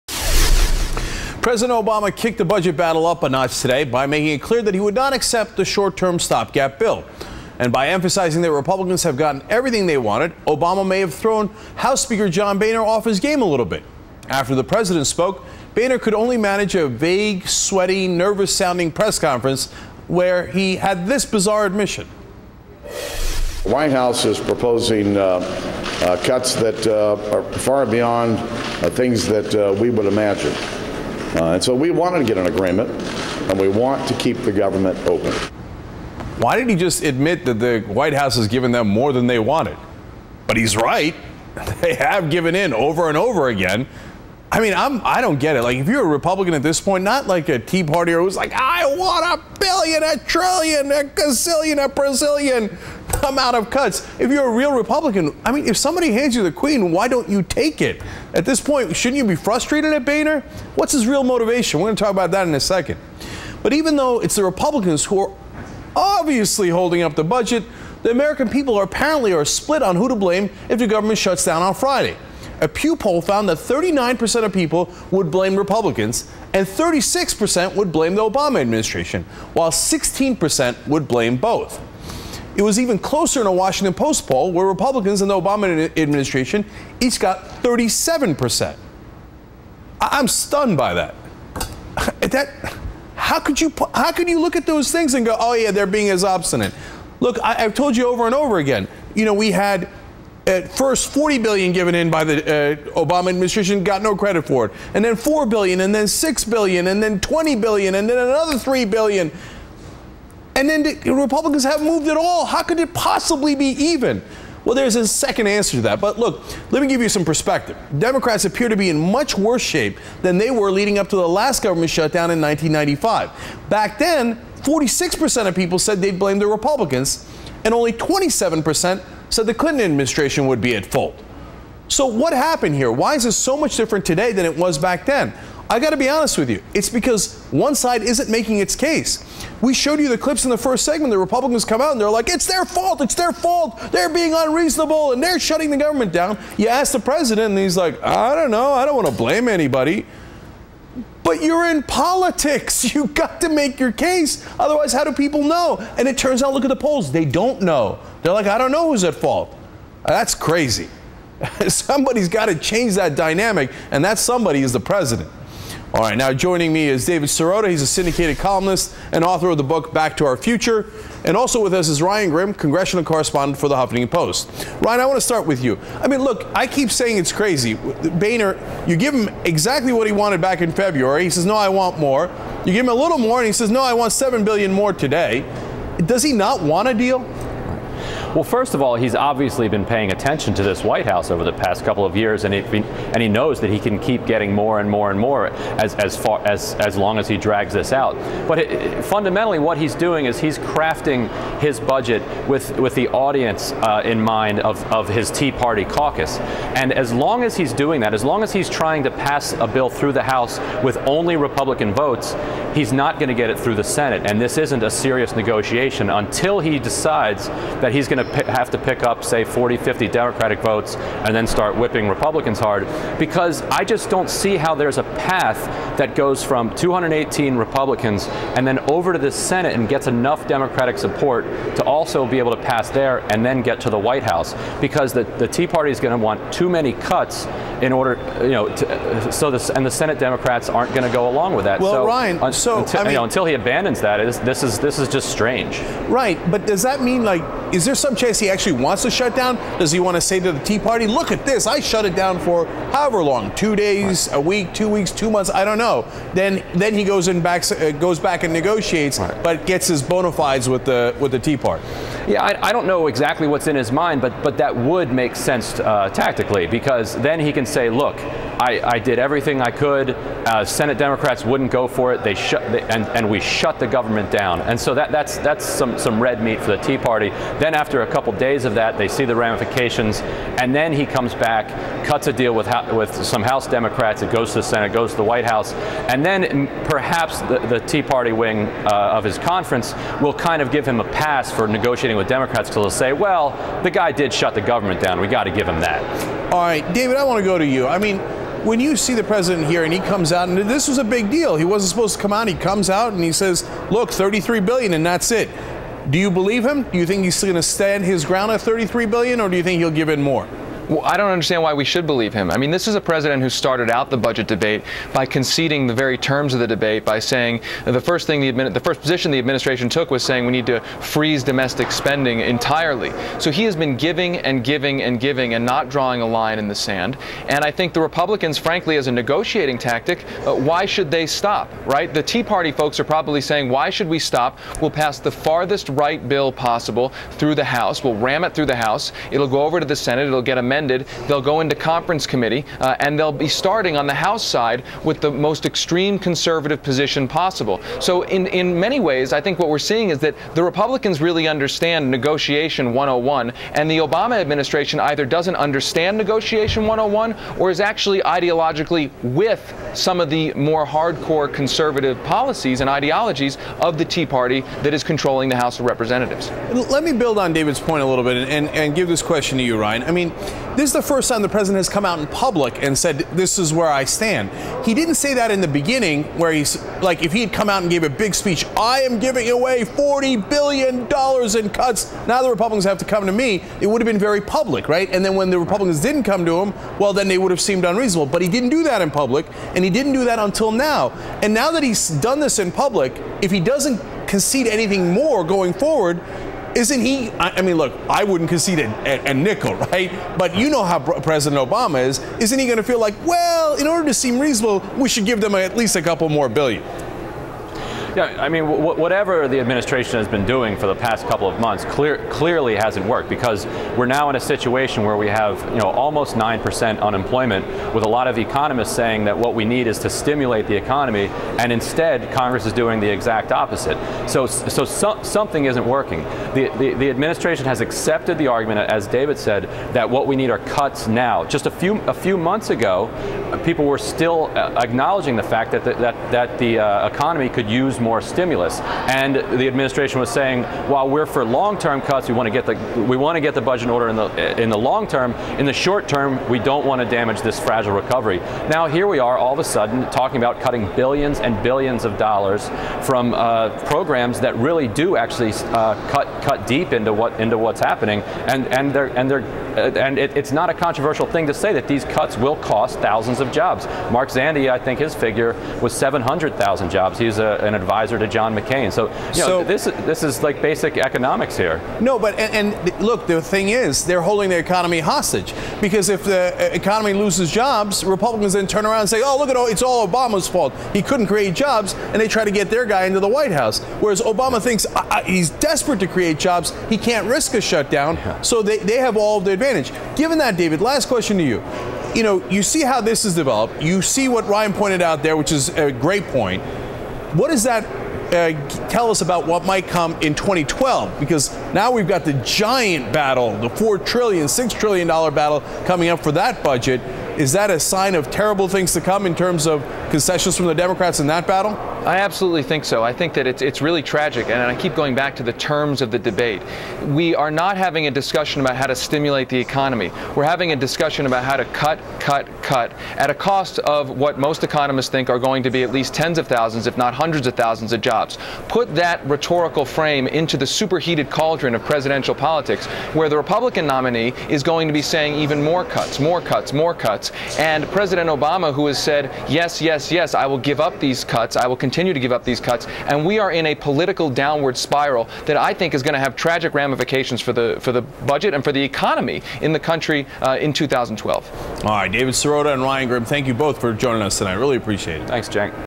Government Shutdown - Tea Party, Obama & Boehner
MSNBC host Cenk Uygur speaks with progressive talk show host and columnist David Sirota and Ryan Grim of the Huffington Post  about budget negotiations and a possible government shutdown.